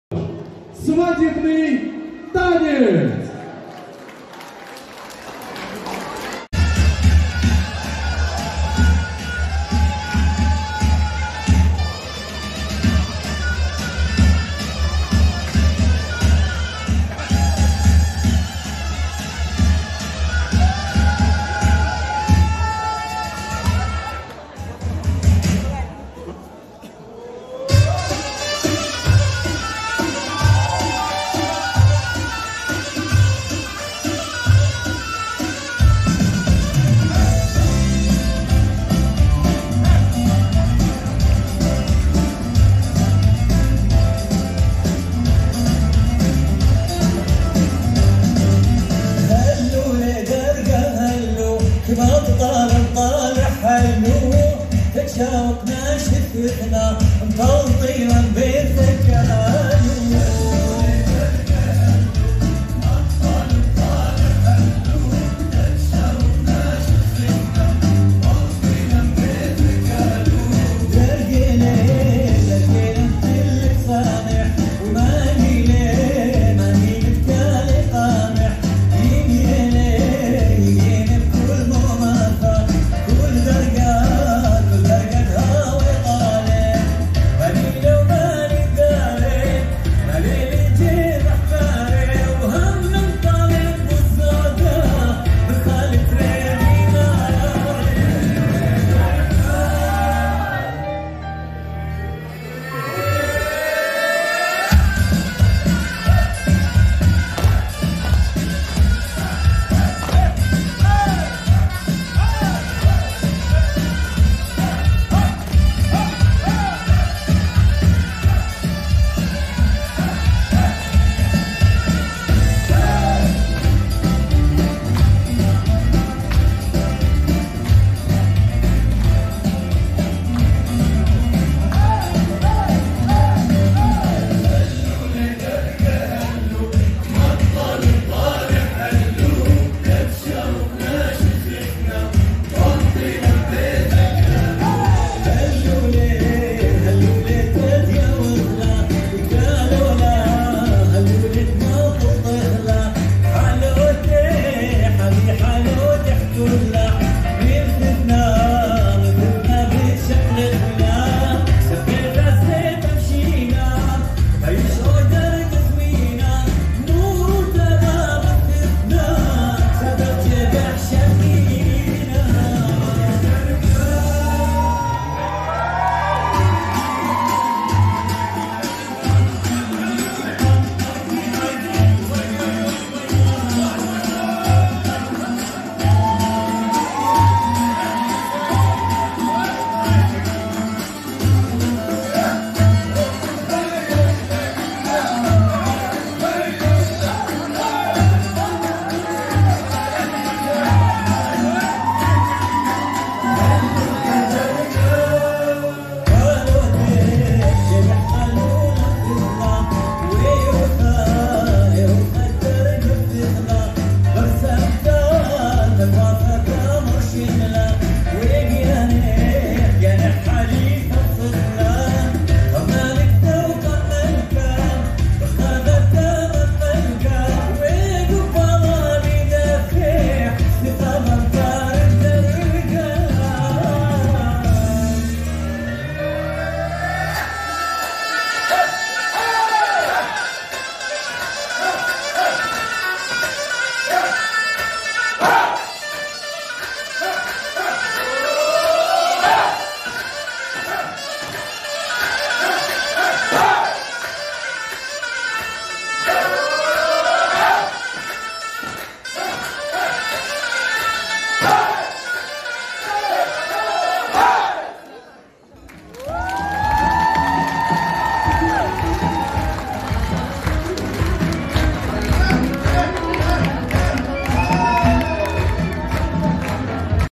Ассирийский ансамбль "Иштар" г.Москва.🎊Assyrian dance group ISHTAR of Moscow.Premiere.🎊 🇷🇺29.10.2022
Премьера.Свадебный танец.Поёт Талал Грейш.
Premiere.Wedding dance.Talal Graish sings.Darga.